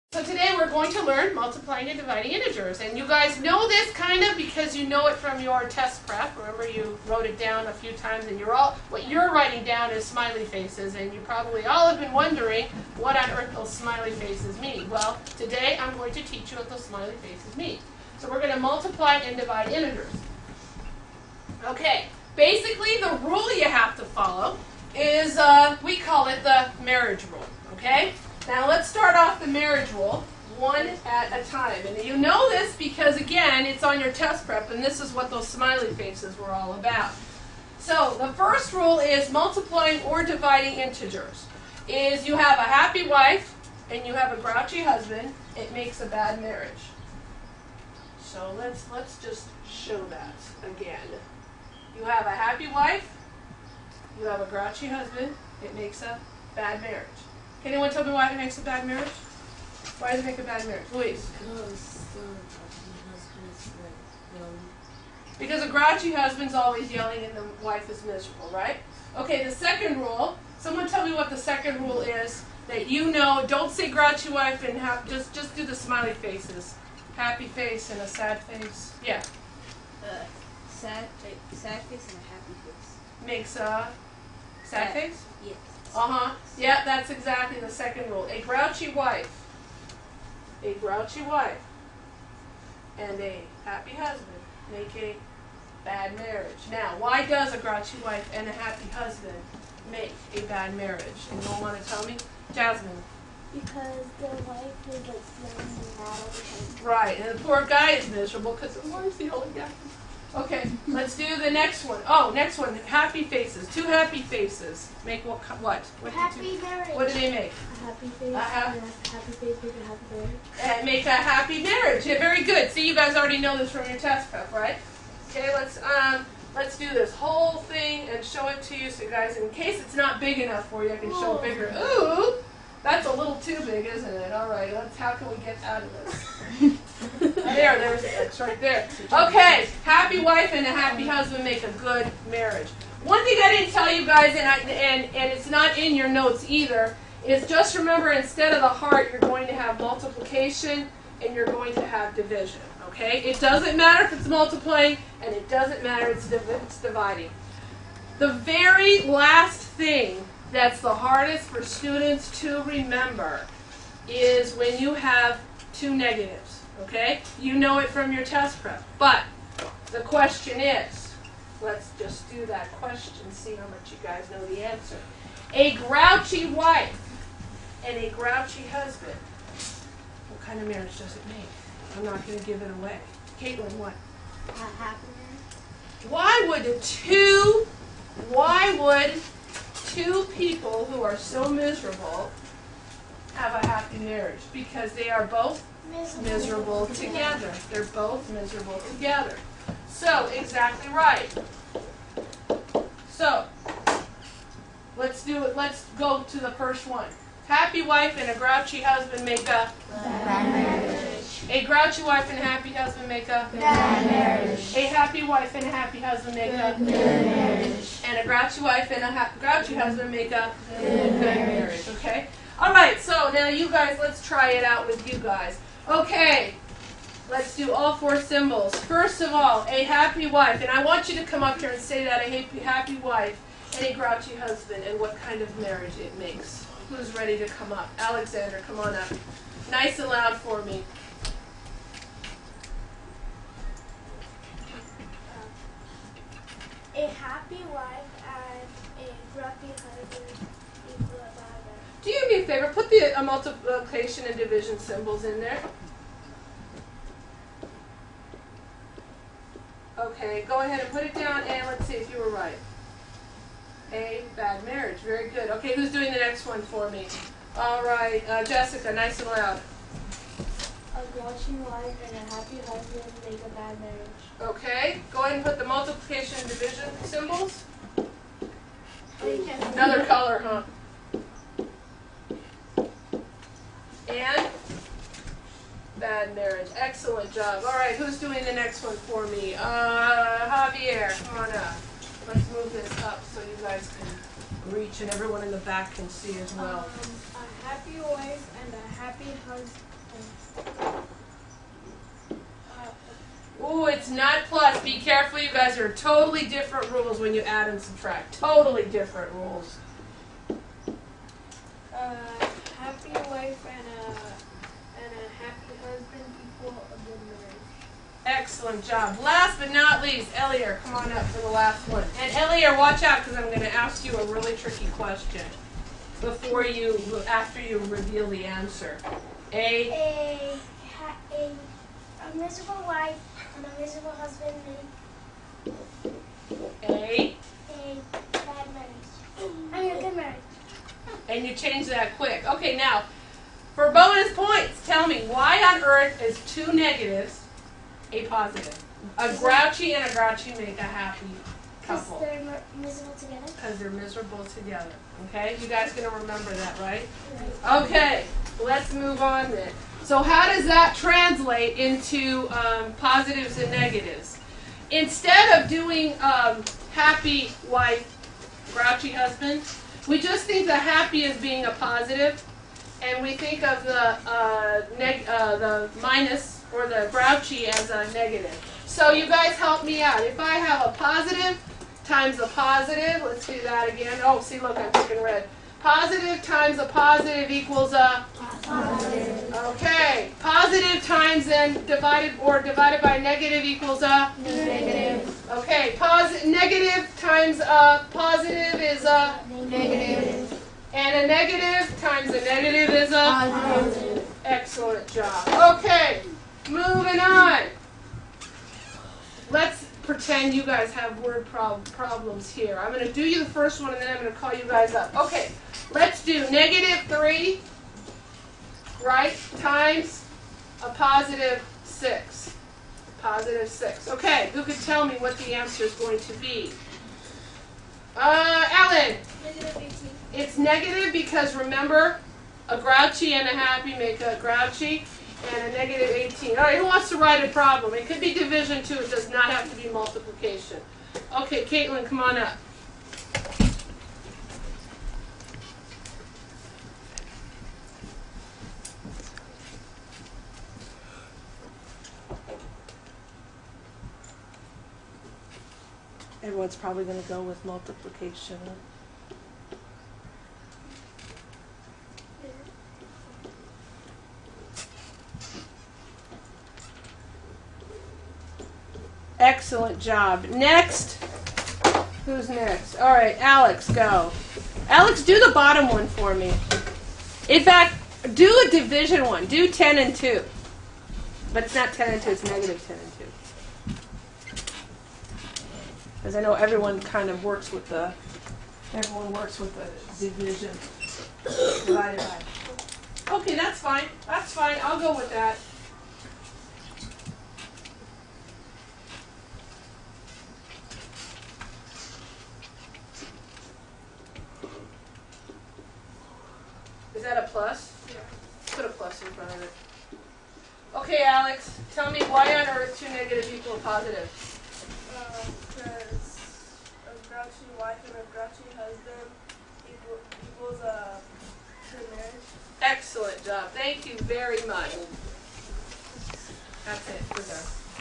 Multiplying and Dividing Integers
TeacherTube User: Dorothyjoh

Multiplying and Dividing Integers using the quotmarriagequot rule on Smartboard